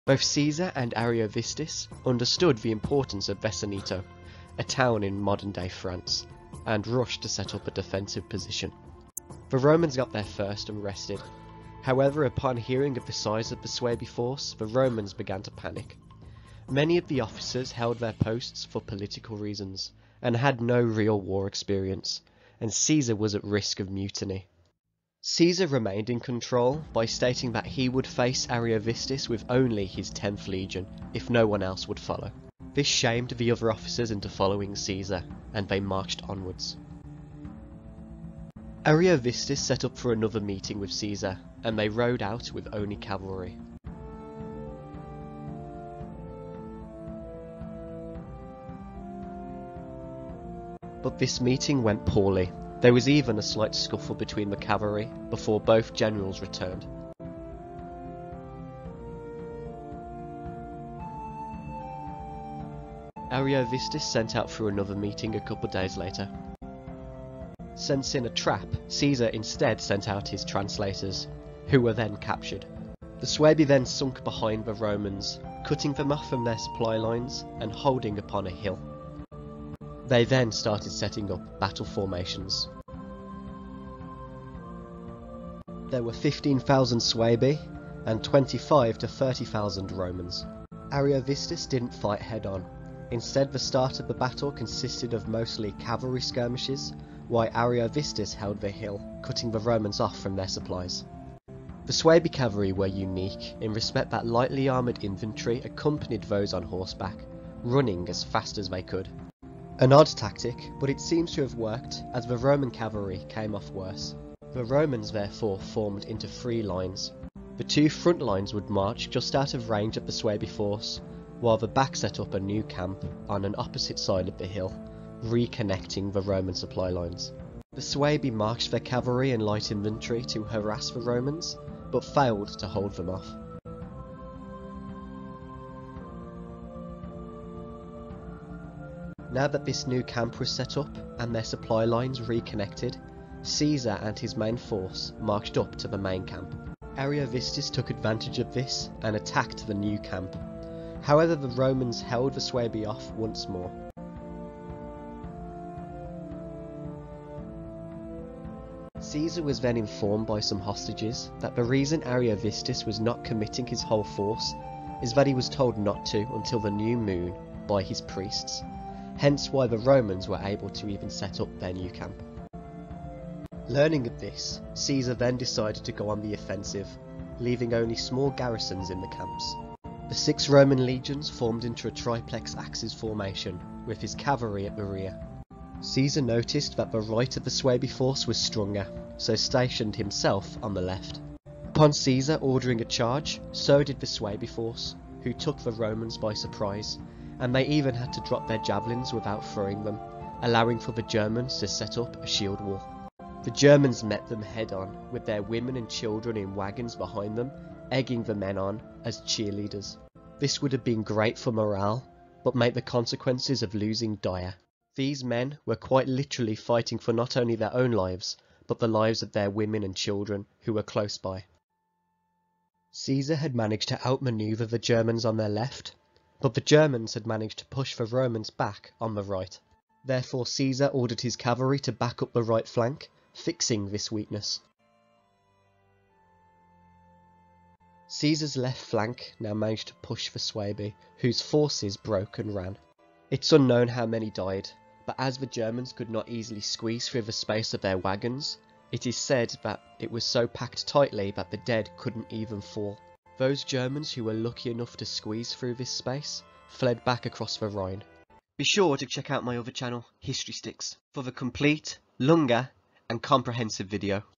Battle of Vosges| Quick and Simple
A Quick and Simple guide to the Battle of Vosges (58 BC): Journey back to the ancient world as we explore the Battle of Vosges, a significant conflict during Julius Caesar's Gallic Wars. Discover the clash between the Roman legions led by Julius Caesar and the Gaulish tribes under the leadership of Ariovistus. Learn about the strategic location of the Vosges Mountains and the fierce fighting that took place. This guide offers a concise overview of the battle's impact on Roman expansion into Gaul and its role in shaping the broader Gallic Wars. Witness a key moment in Caesar's military campaign and its historical implications.